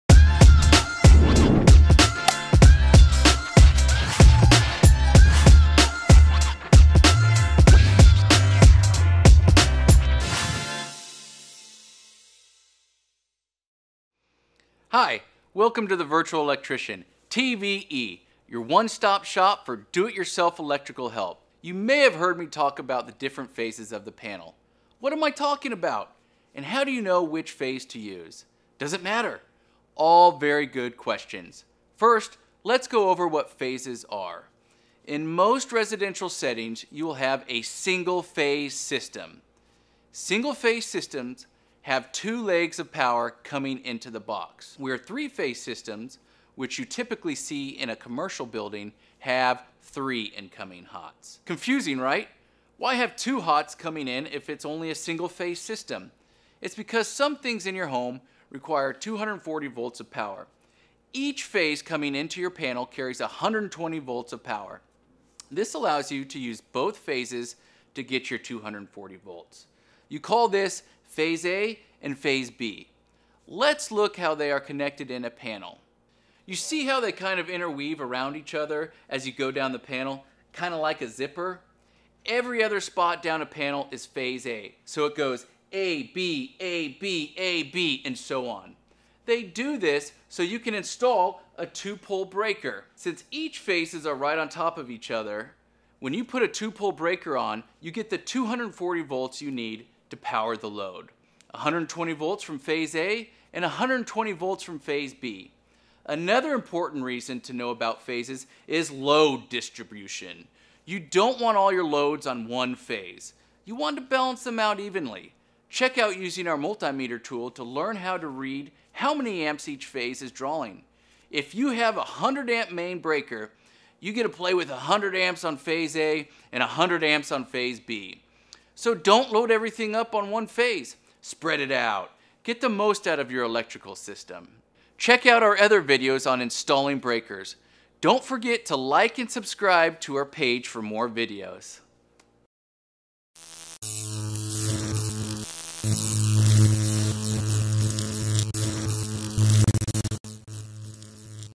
The Different Phases Of A Breaker Box
Every residential house has 2 incoming legs of power connected to their electrical panel.  This is called a single phase system.  The legs of power are called phase A and phase B. Each leg carries 120 volts of power. You need 2 legs of power because there are appliances in your home that require 240 volts to operate.  The 2 phases interweave each other in the electrical panel.  You must install 2 pole breakers to power these appliances.  Another type of electrical system is the three phase system.  Three phase systems are found in commercial buildings.  When installing electrical circuits in your breaker box you want to balance phase A and phase B evenly.  This will produce a more efficient electrical system. Performing a load calculation will help you balance your electrical loads.  and schedule a virtual electrician to answer all your questions.